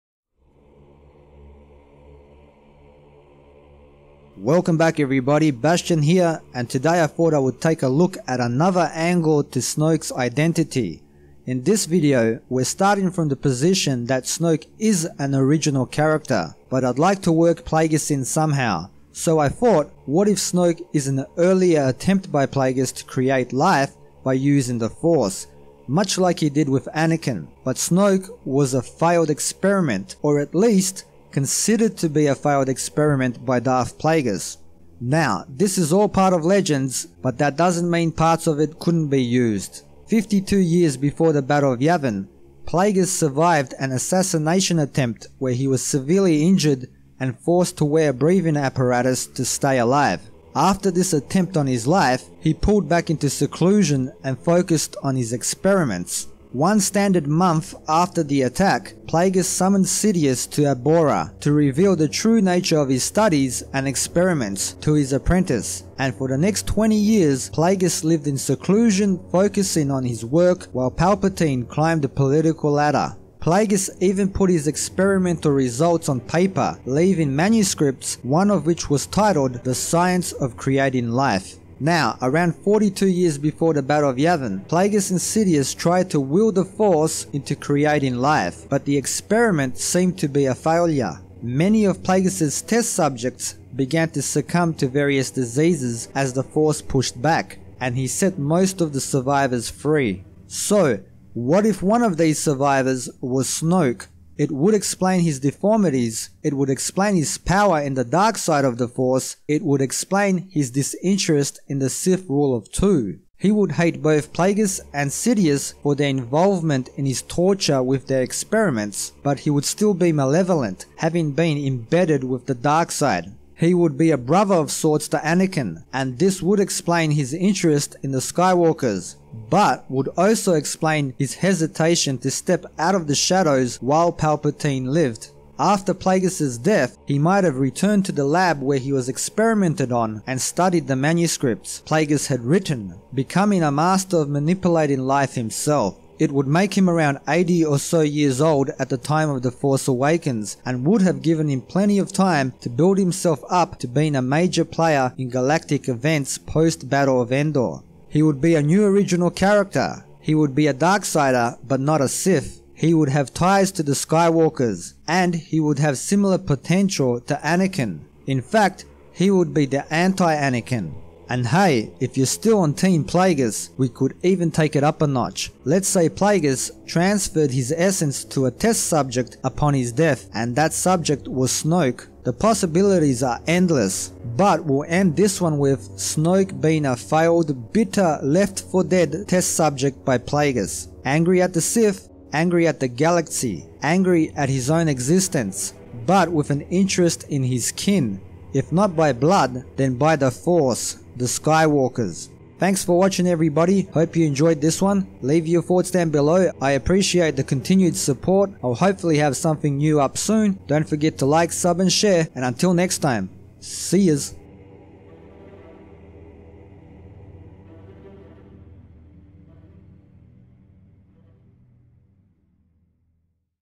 Snoke A Failed Experiment By Plagueis!?! [STAR WARS]
Another Snoke theory. You know you love them :) This one explores the possibility that Snoke was a failed experiment by Darth Plagueis and that somehow he managed to survive.